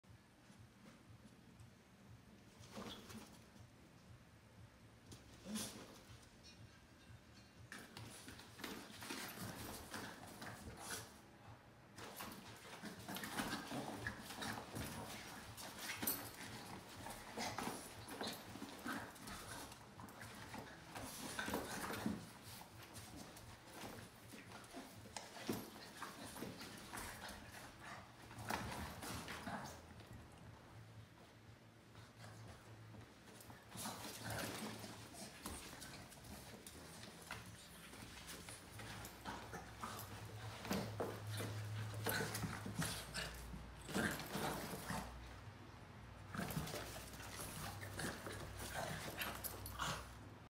BOSTON TERRIERS | Sibling Rivalry | Two dogs are more fun than one
100mph or 0mph there is no in between.  These two are constantly egging each other on and playing non stop.

Do you have Bostons that do this too?

These two have a PhD in being clowns.